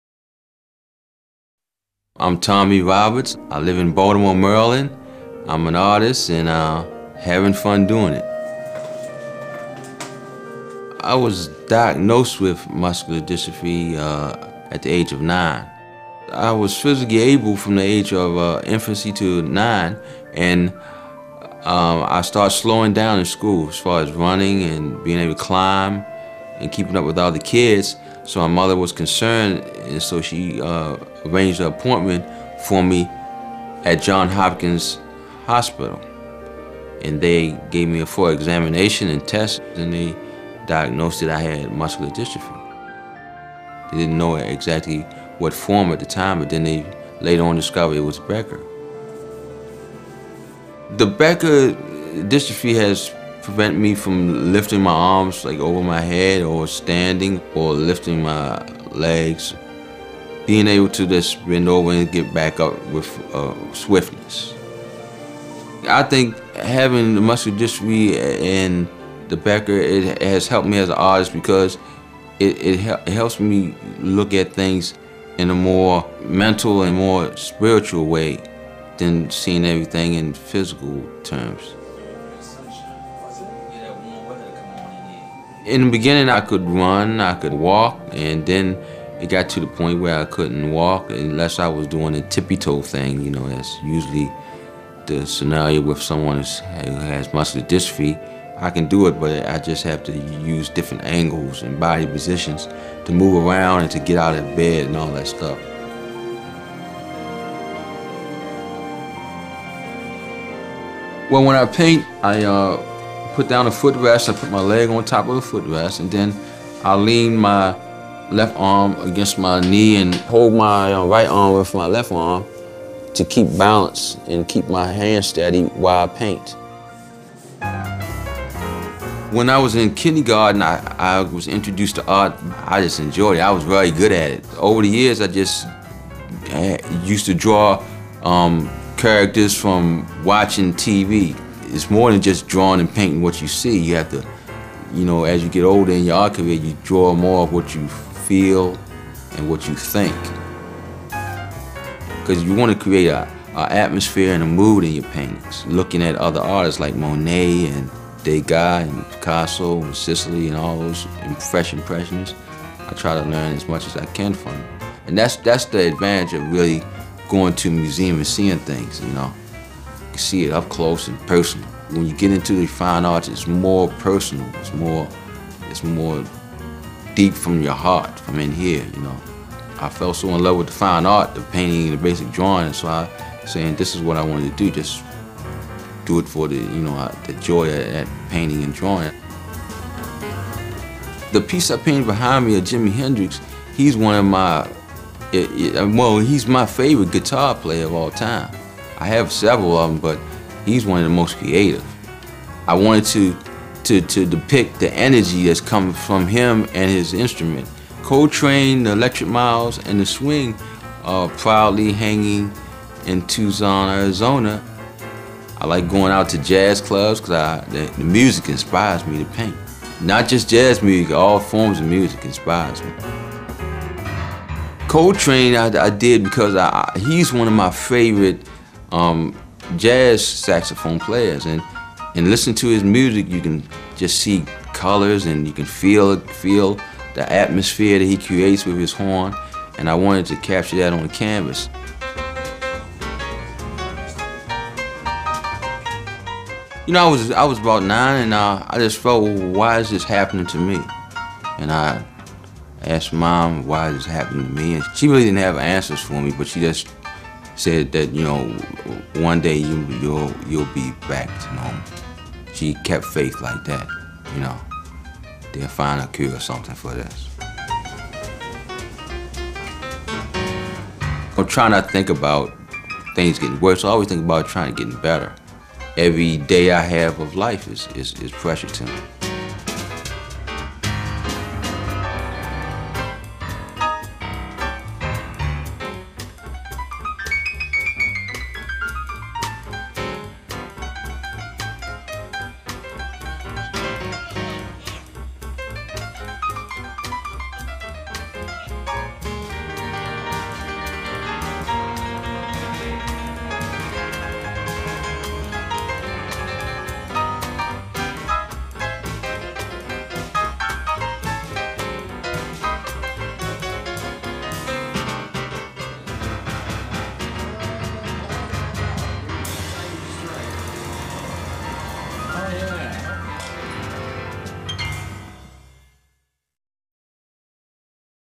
Tommy Roberts Interview
Tommy Roberts Interview from his studio featured on the Muscular Dystrophy Association Telethon December 21, 2010